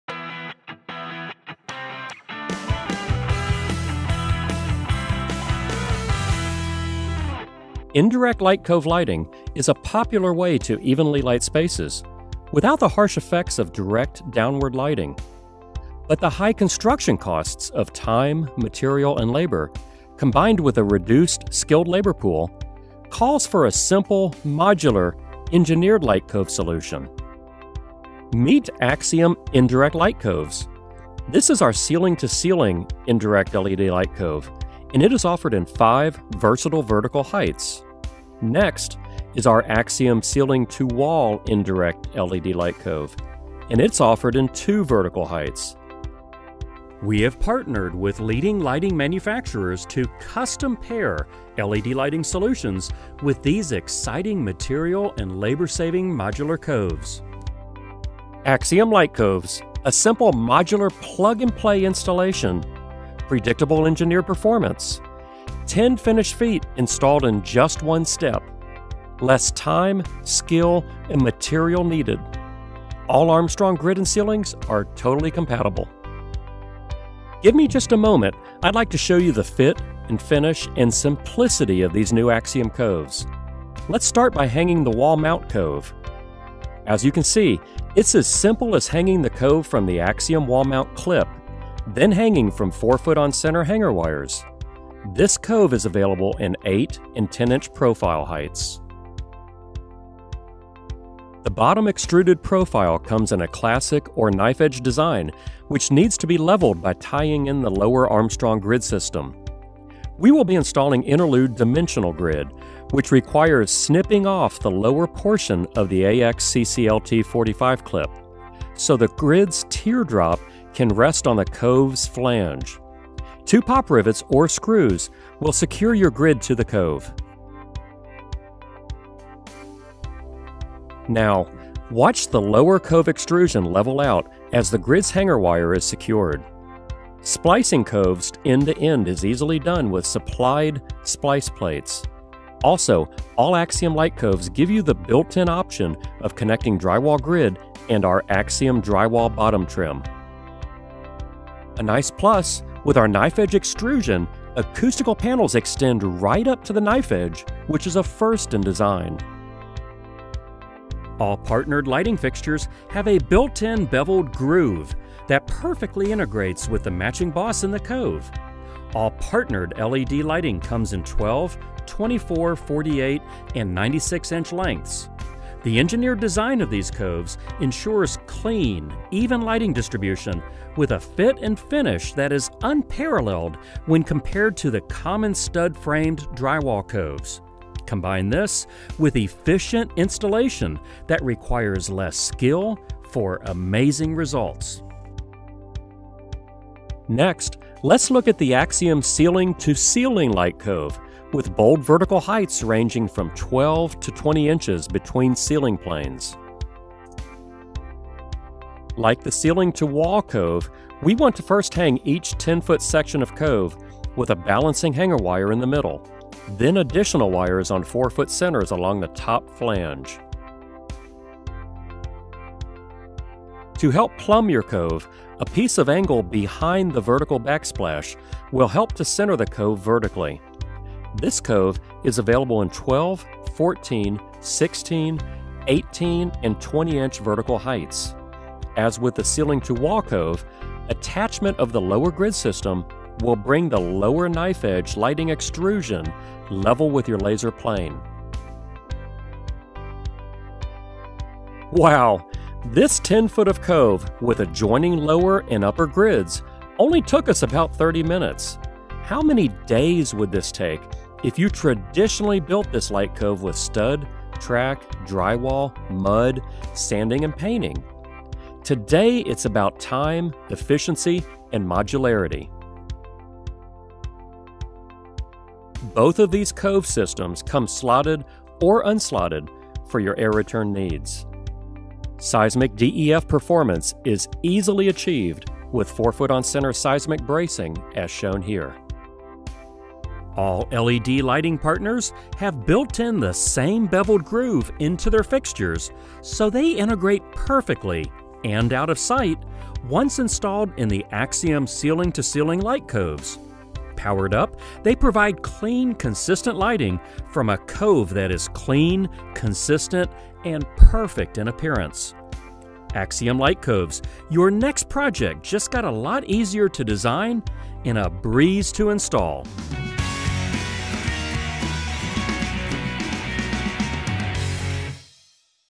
Ceiling Indirect Lighting Using Drywall Grid | Installation How to | Armstrong Ceiling Solutions
See how to install indirect lighting using drywall grid in this 5-minute installation video from Armstrong Ceiling Solutions. AXIOM Indirect Light Coves are a simple, modular engineered light cove solution.

The benefits of AXIOM Indirect Light Coves include:
* Modular plug and play
* Predictable engineered performance
* 10 finished feet installed in one step
* Less time, skill, and material needed
* Armstrong grid/ceiling compatibility

With AXIOM Light Coves, your next project just got a lot easier to design and a breeze to install.